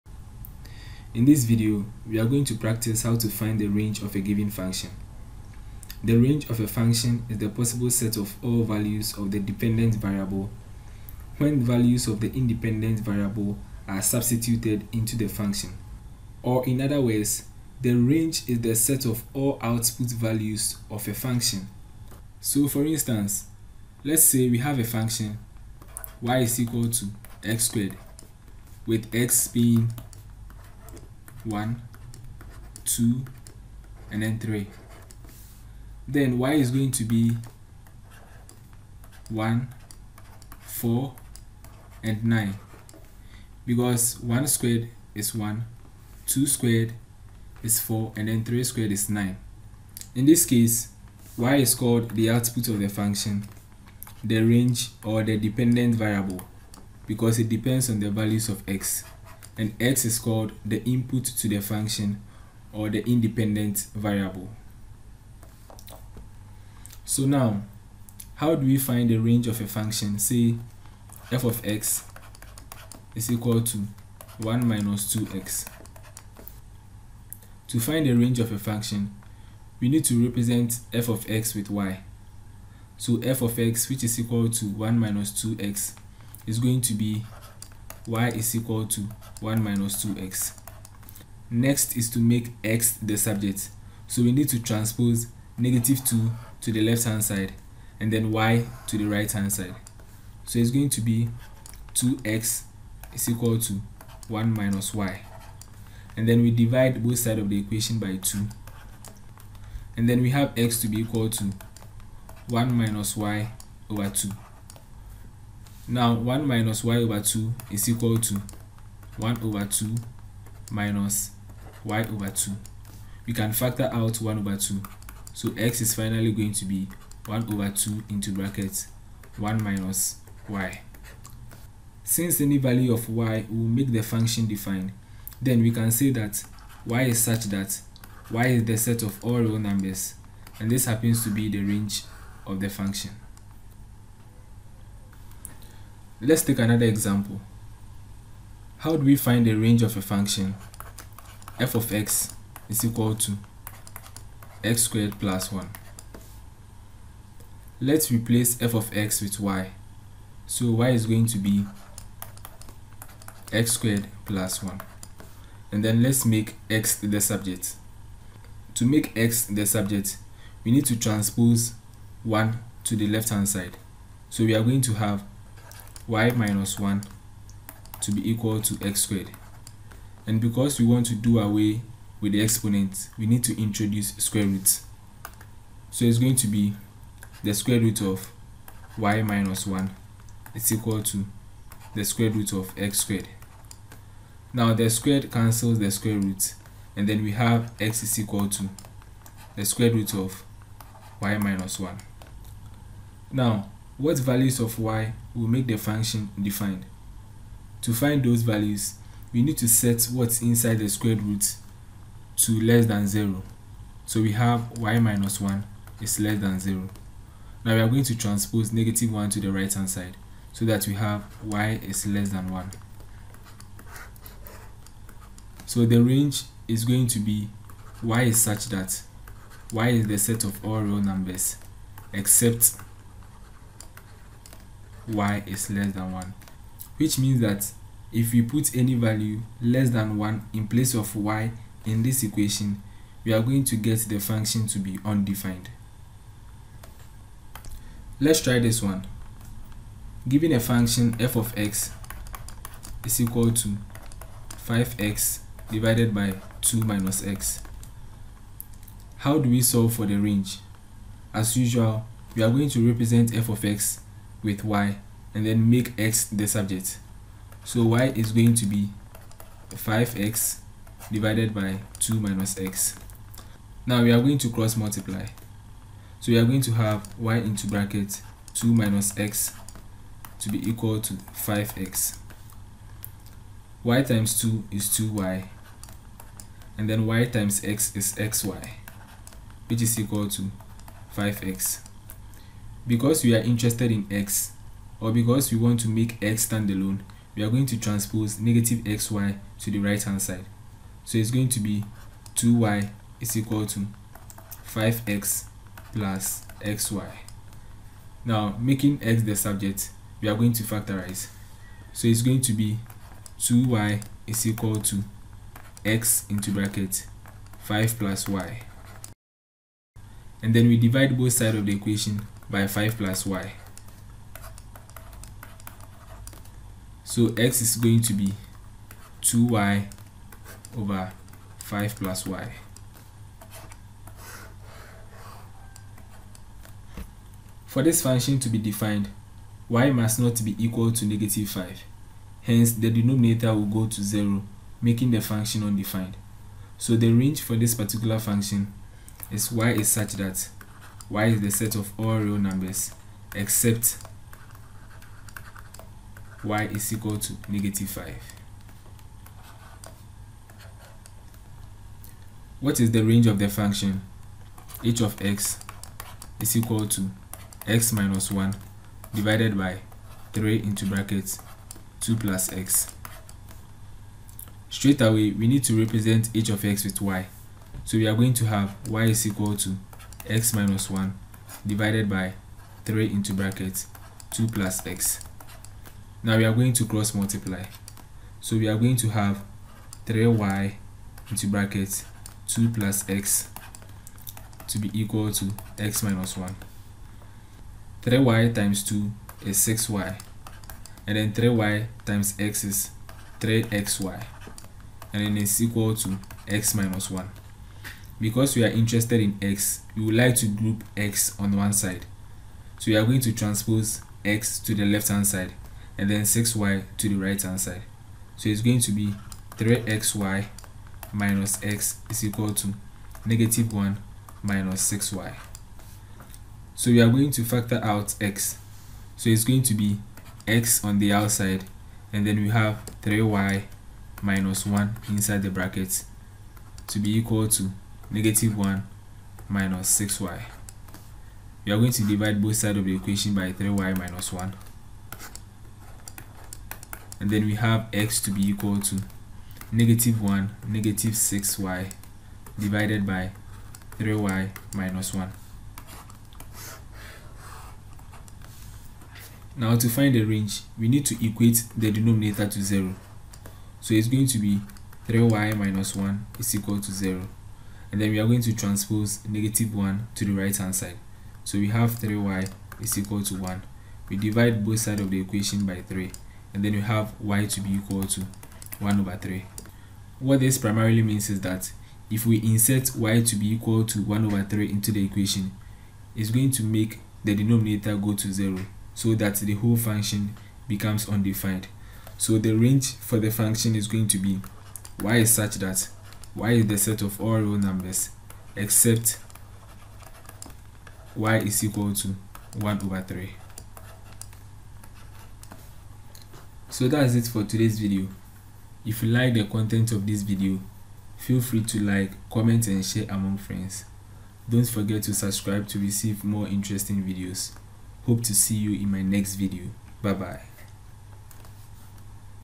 How to Find the Range of a function | SHS 1 ELECTIVE MATH
How to Find the Range of a function - Rational, Square roots

This video teaches how you can find the range of a function. The range of a function is the values of the dependent variable (y) that is obtained as a result of applying inputs or (x) values into the function. The (x) values are the independent variables.

Content
1. How to find the range of a linear function
2. How to find the range of a function with a radical
3. How to find the range of a rational function.

Ghana
For all Ghanaian Senior High School Students
Elective Maths